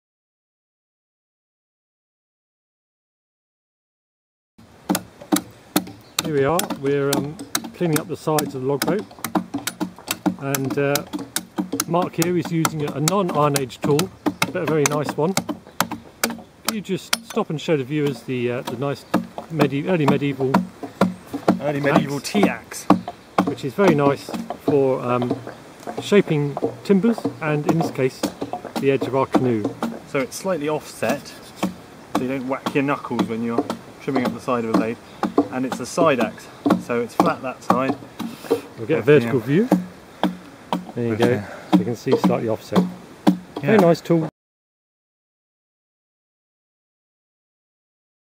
Paddling through Prehistory Video 6
The Story of our Dugout Canoe.
The sixth installment of our video diary following the construction of a logboat at the Avalon Marshes Centre, based on a local Iron Age example, the Shapwick Canoe.

Marc Cox shows off an early medieval-style t-axe used on our canoe to finish the sides.

to follow our adventures and get involved!